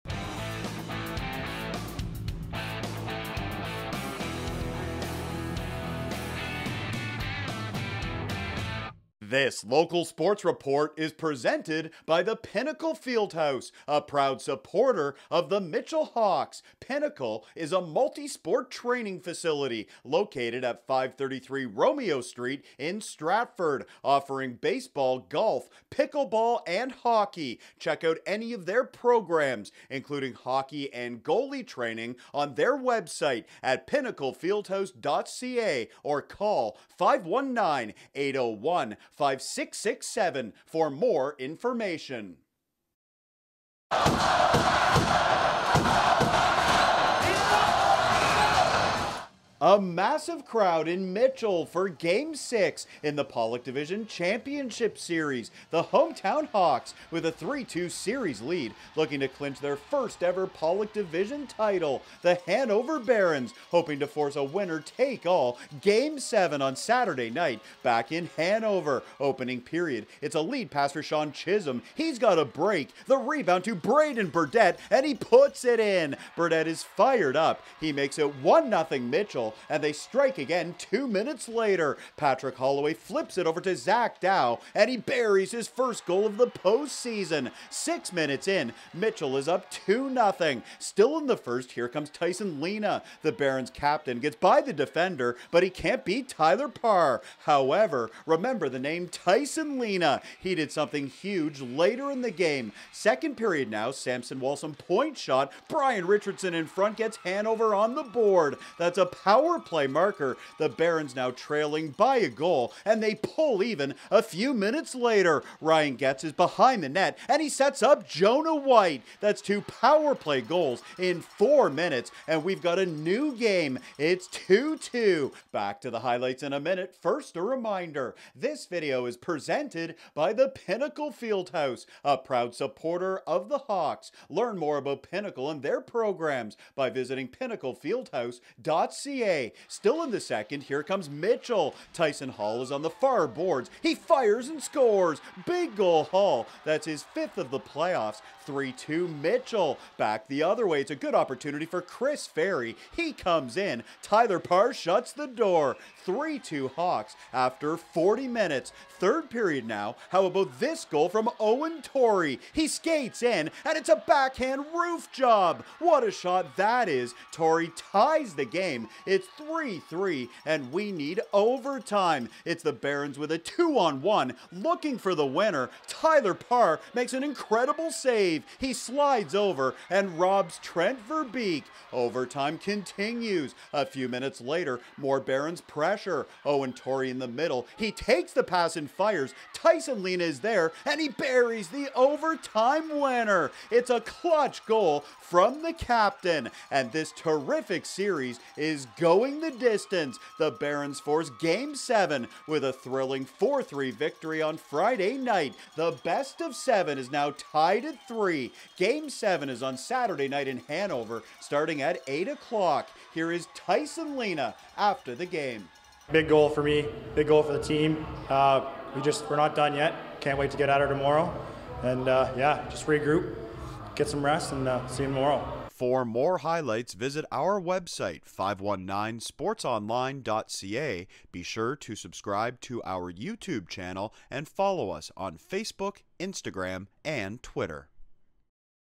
Pollock Division Championship (Game 6) - Hanover Barons vs Mitchell Hawks
Highlights from Friday April 16th between the Barons and Hawks in Mitchell.

This video is presented by the Pinnacle Fieldhouse at 533 Romeo Street in Stratford. Learn more about Pinnacle and their programs by visiting